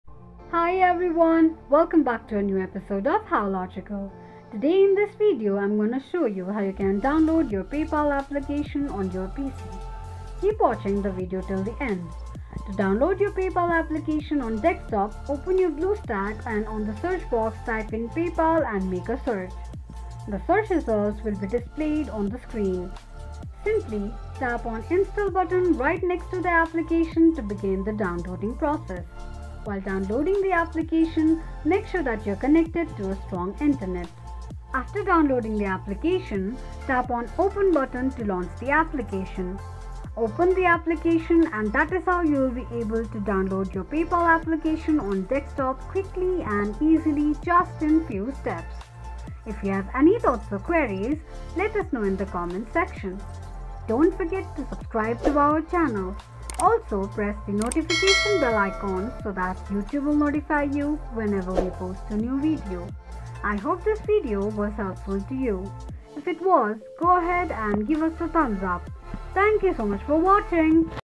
How to Download & Install PayPal on Desktop PC 2024?
In this step-by-step video tutorial, you'll learn how to download PayPal on your PC. With PayPal, you can easily send and receive money online, shop at millions of merchants, and more.